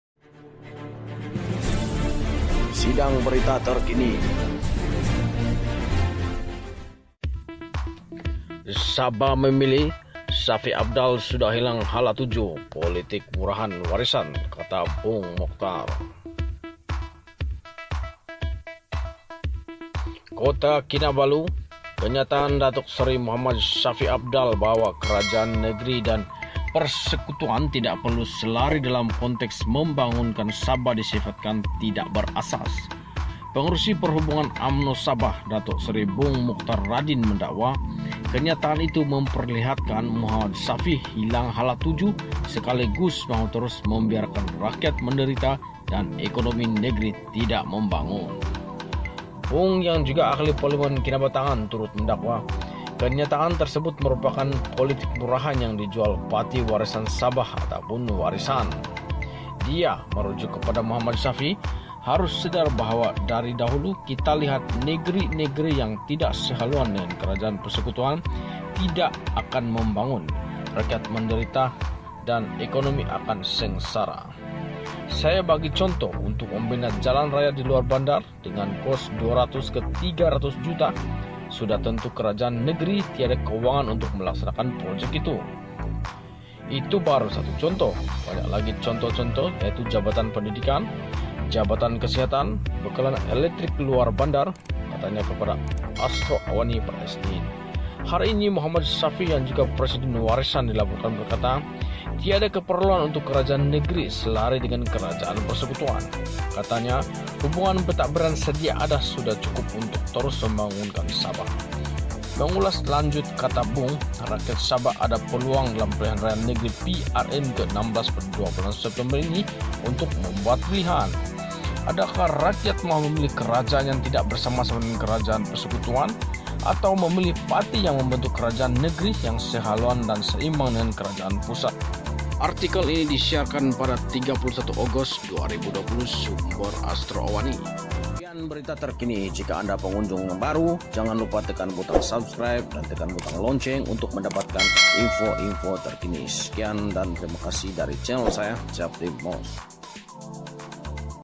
Shafie Apdal Hilang Hala Tuju. Politik Murahan
KOTA KINABALU: Kenyataan Datuk Seri Mohd Shafie Apdal bahawa kerajaan negeri dan Persekutusan tidak perlu selari dalam konteks membangunkan Sabah disifatkan tidak berasas.

Pengerusi Perhubungan UMNO Sabah, Datuk Seri Bung Moktar Radin mendakwa, kenyataan itu memperlihatkan Mohd Shafie hilang hala tuju sekali gus mahu terus membiarkan rakyat menderita dan ekonomi negeri tidak membangun.

Bung yang juga Ahli Parlimen Kinabatangan turut mendakwa, kenyataan tersebut merupakan politik murahan yang dijual Parti Warisan Sabah (Warisan).

"Dia (Mohd Shafie) harus sedar bahawa dari dahulu kita lihat negeri-negeri yang tidak sehaluan dengan kerajaan Persekutuan tidak akan membangun, rakyat menderita dan ekonomi akan sengsara.

"Saya bagi contoh, untuk membina jalan raya di luar bandar dengan kos RM200 ke RM300 juta, sudah tentu kerajaan negeri tiada kewangan untuk melaksanakan projek itu.

"Itu baru satu contoh. Banyak lagi contoh-contoh (iaitu) Jabatan Pendidikan, Jabatan Kesihatan, bekalan elektrik luar bandar," katanya kepada Astro AWANI pada Isnin.

Hari ini, Mohd Shafie yang juga Presiden Warisan dilaporkan berkata, tiada keperluan untuk kerajaan negeri selari dengan kerajaan Persekutuan.

Katanya, hubungan pentadbiran sedia ada sudah cukup untuk terus membangunkan Sabah.

Mengulas lanjut, kata Bung, rakyat Sabah ada peluang dalam Pilihan Raya Negeri (PRN) ke-16 pada 26 September ini untuk membuat pilihan.

"Adakah rakyat mahu memilih kerajaan yang tidak bersama-sama dengan kerajaan Persekutuan atau memilih parti yang membentuk kerajaan negeri yang sehaluan dan seimbang dengan kerajaan Pusat," katanya.